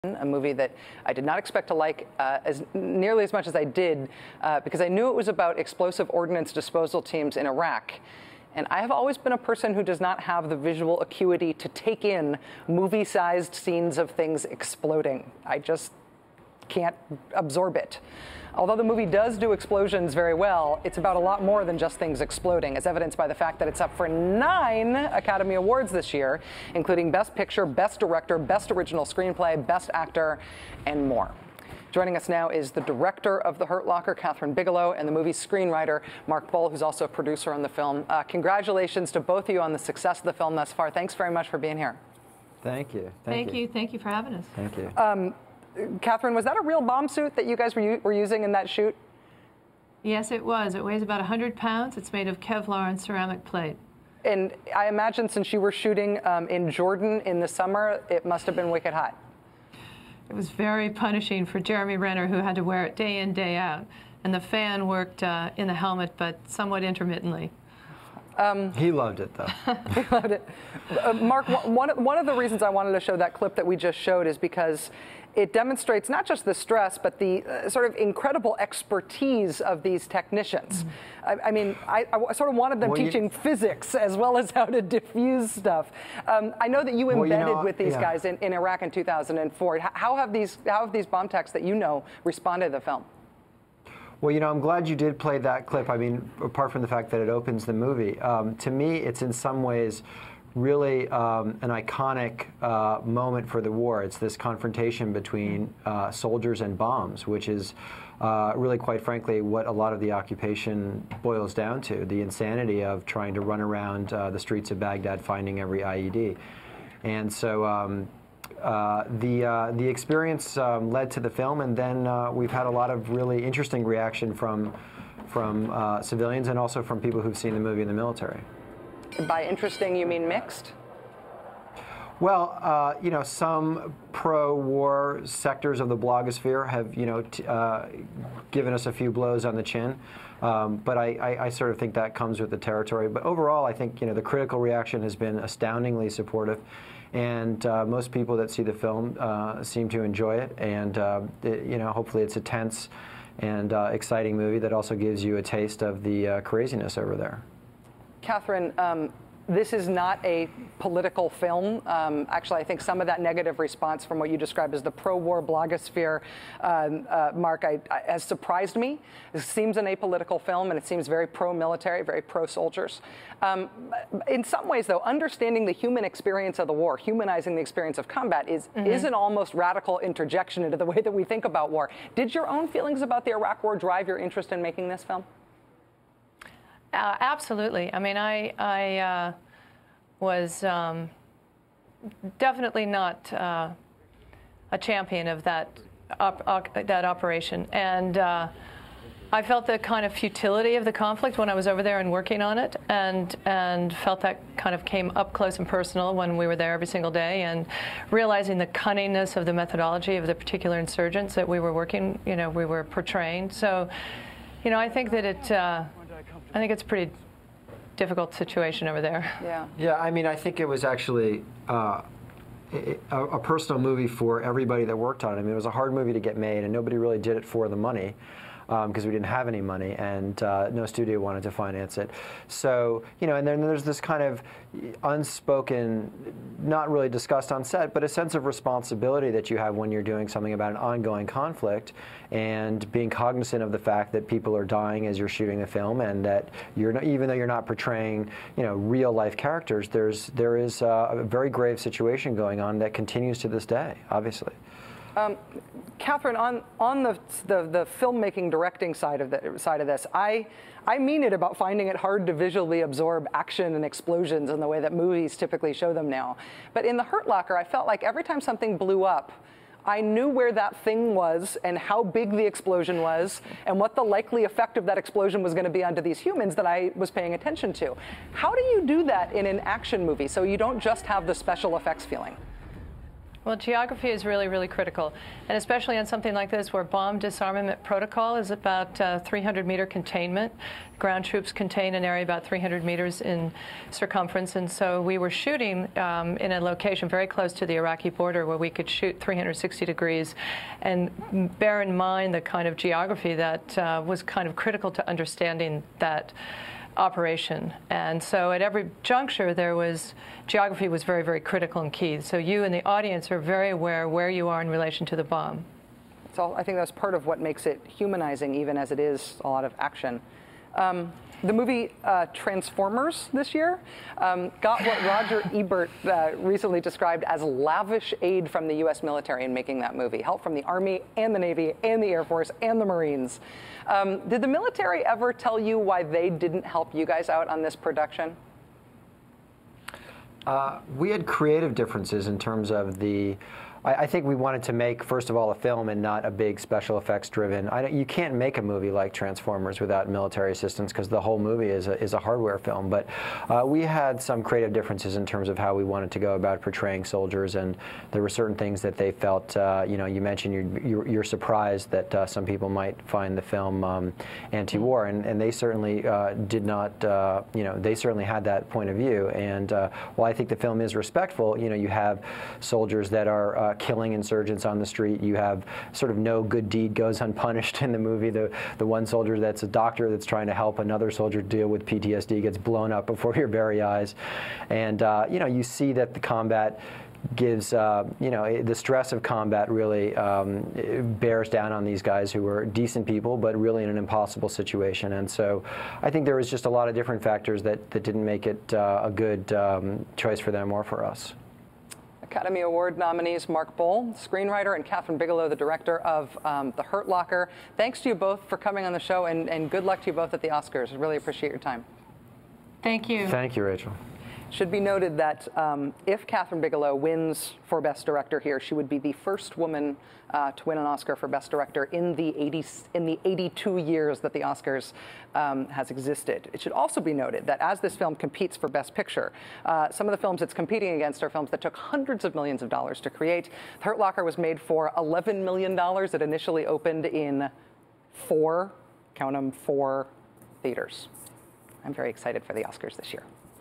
Kathryn Bigelow Interview With Rachel Maddow
Academy Award winning director Kathryn Bigelow talks with Rachel Maddow about her Academy Award winning film The Hurt Locker.